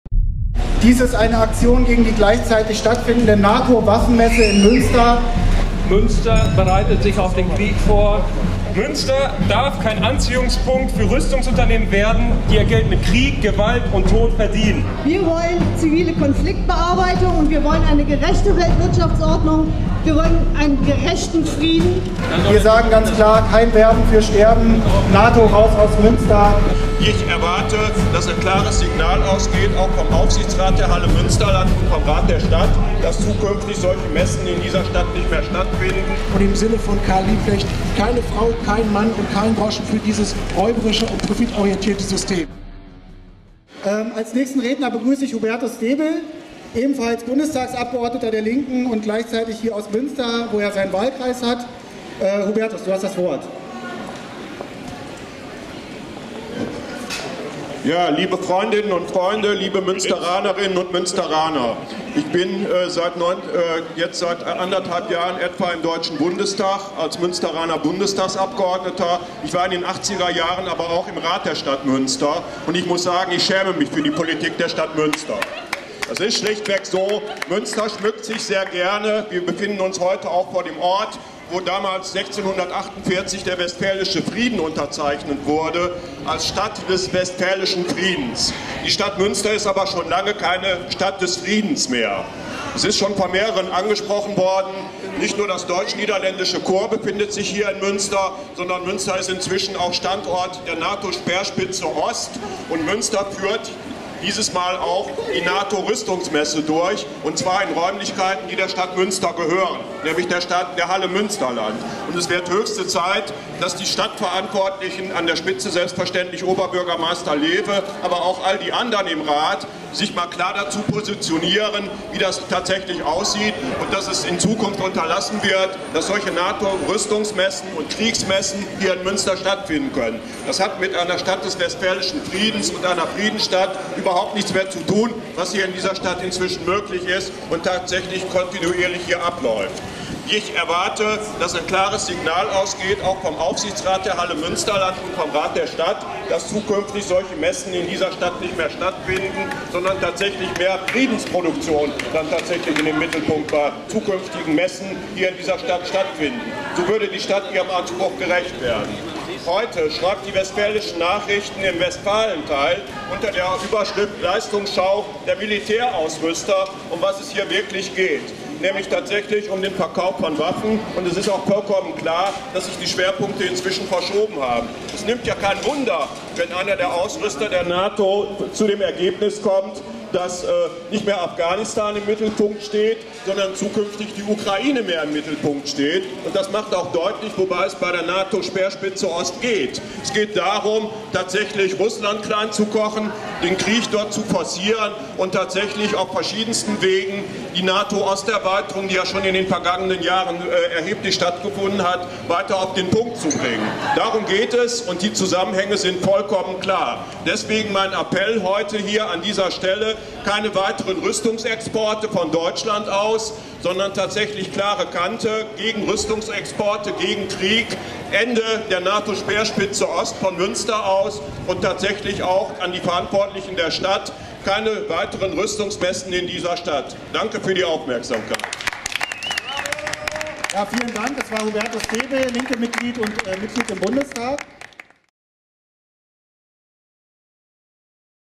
Rückblick 2015: Protest in Münster gegen Nato-Waffenmesse - Rede Hubertus Zdebel (MdB DIE LINKE.)
Münster vor 5 Jahren (15. April 2015): Protest - Kundgebung  mit mehreren Redner*innen gegen   die NATO Agentur “NATO Support Agency” (NSPA) - Waffenmesse in der Halle Münsterland, die vom vom 14. bis 16. April 2015 stattfand.
Unter dem Motto “Neue Märkte, neue Zielgruppen” hatte die Messe mit dem Namen “Defense Expo” zeitgleich mit der Polizeimesse “Ipomex” stattgefunden.